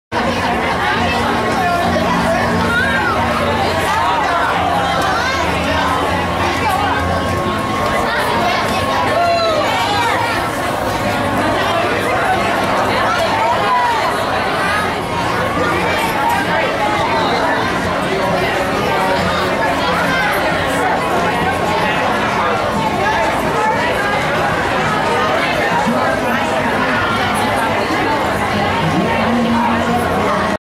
Girl Scout Pavilion at Cookie Classic
Girls, families and community program partners enjoy an active day in the Girl Scout Pavilion during Girl Scouts of Greater Chicago and Northwest Indiana's Cookie Classic/100th Birthday Bash with a Mission.